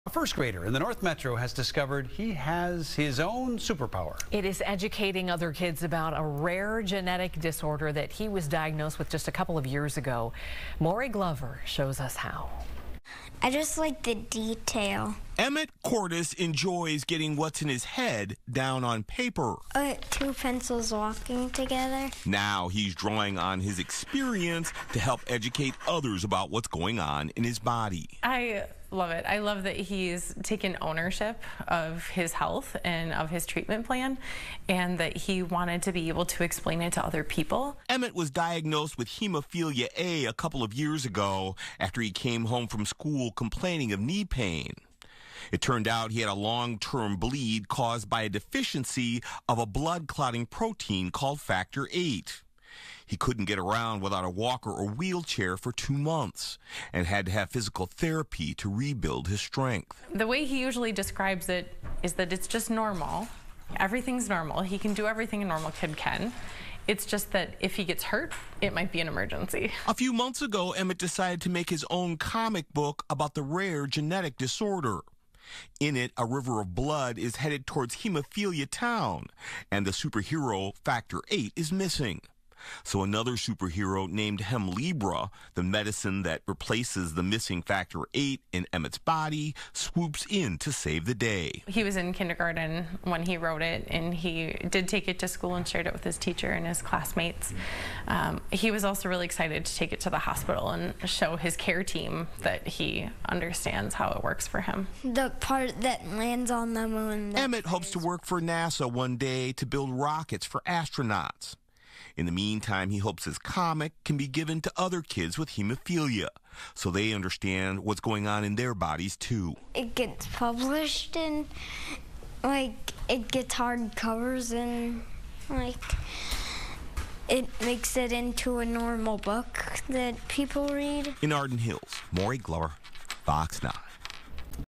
Kids comic book highlights rare disorder
Emmett Cordes enjoys getting what's in his head down on paper, and he's drawing on his experience to help educate others about what is going on in his body.